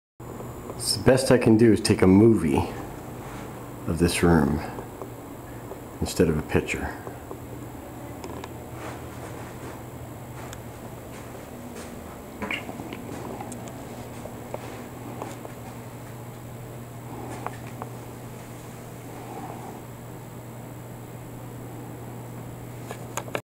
My Art at cpc
one of the rooms at cpc that I paintes all the walls and cieling